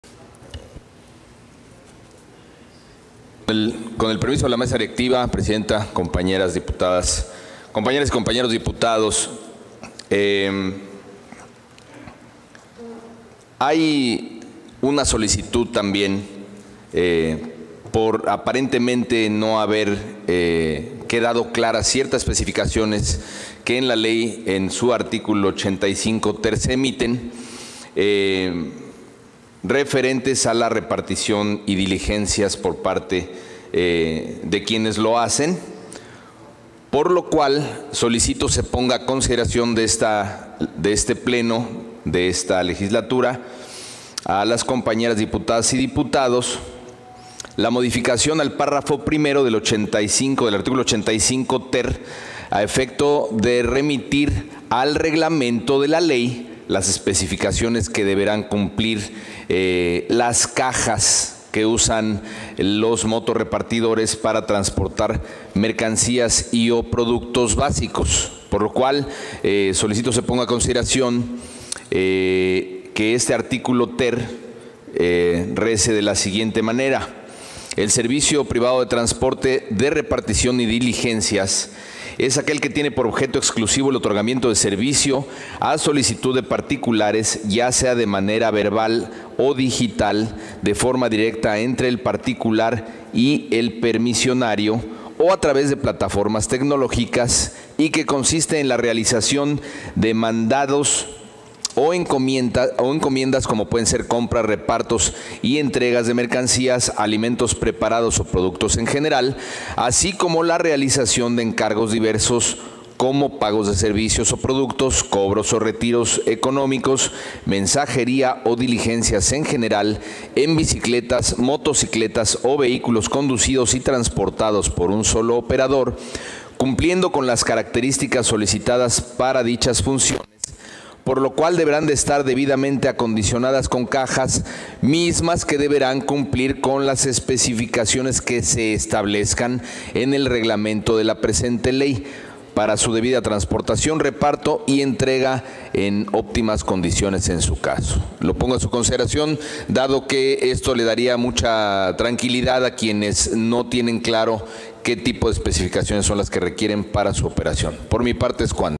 Intervención Dip. Luis Humberto Aldana Navarro. MORENA (16/Febrero/2023)
Intervención del Dip. Luis Humberto Aldana Navarro, de MORENA, en relación al Dictamen con proyecto de decreto por el que se reforman, adicionan y derogan diversas disposiciones de la Ley de Movilidad del Estado de Quintana Roo; para su aprobación, en su caso. Publicado en la Gaceta Parlamentaria Año I, número 26 Ordinaria, de fecha 24 enero de 2023.